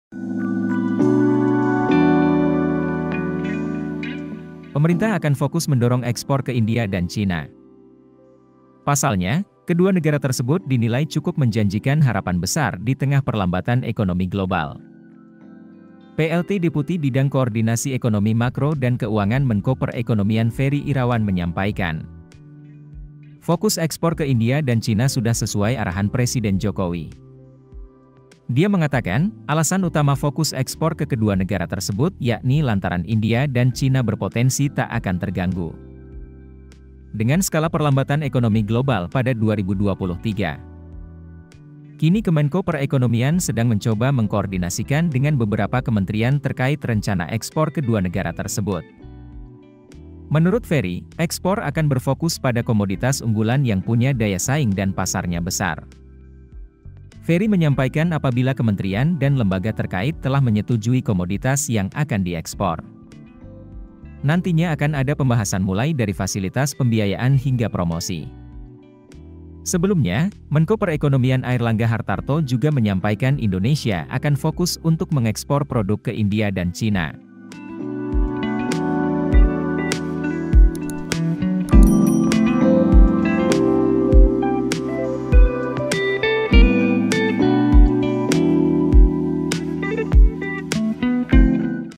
Pemerintah akan Mendorong Ekspor ke China dan India
Pemerintah akan fokus mendorong ekspor ke India dan China.

Pasalnya, kedua negara tersebut dinilai cukup menjanjikan harapan besar di tengah perlambatan ekonomi global.

Plt Deputi Bidang Koordinasi Ekonomi Makro dan Keuangan Menko Perekonomian Ferry Irawan menyampaikan fokus ekspor ke India dan China sudah sesuai arahan Presiden Jokowi.

Dia mengatakan, alasan utama fokus ekspor ke kedua negara tersebut yakni lantaran India dan China berpotensi tak akan terganggu dengan skala perlambatan ekonomi global pada 2023.

Kini Kemenko Perekonomian sedang mencoba mengkoordinasikan dengan beberapa kementerian terkait rencana ekspor ke dua negara tersebut.